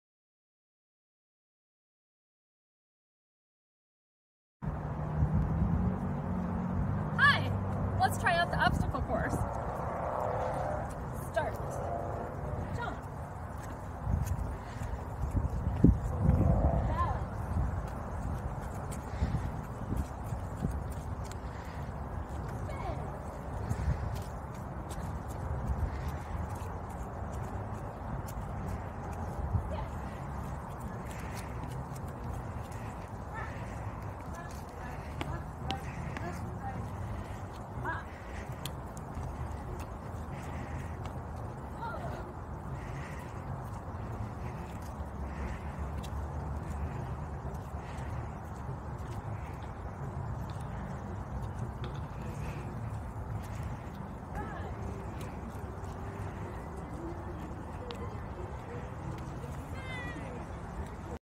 Silly Sidewalk Obstacle Course
Our obstacle course has survived both sun and rain and is still available for you to try out, right in front of the library!  If you complete it, be sure to stop by the youth desk for a bookmark, while supplies last!